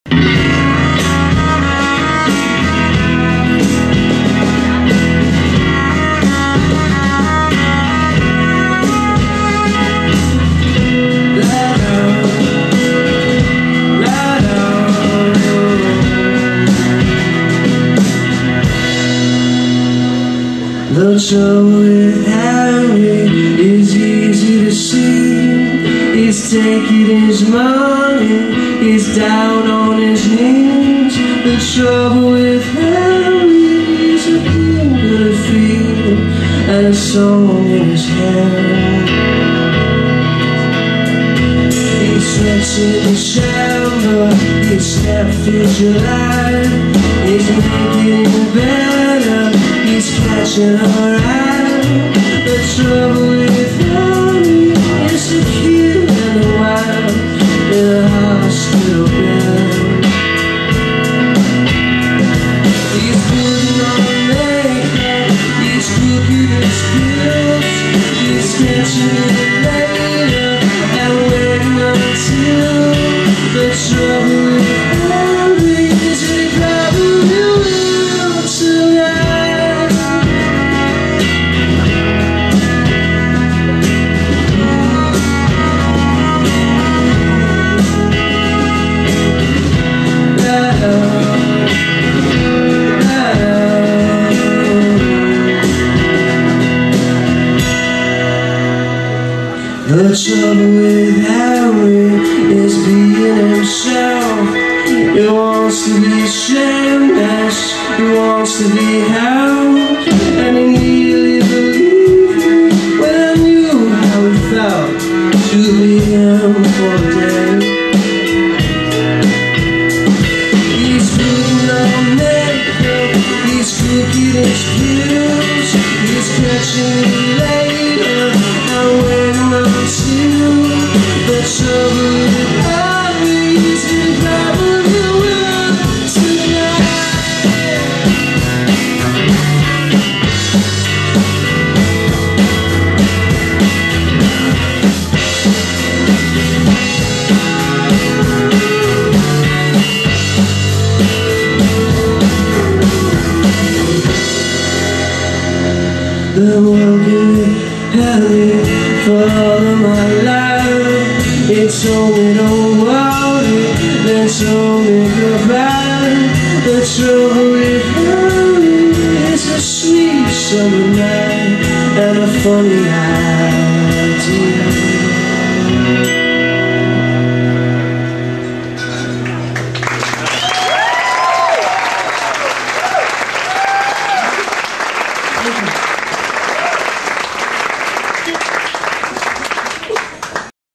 The Trouble With Henry [2006-05-14]
Recorded live at 3rd & Lindsley in Nashville, TN on May 14, 2006. Filmed with permission.

The band: David Mead (guitar/keyboards/ukelele), David Henry (cello), Paul Deakin (drums).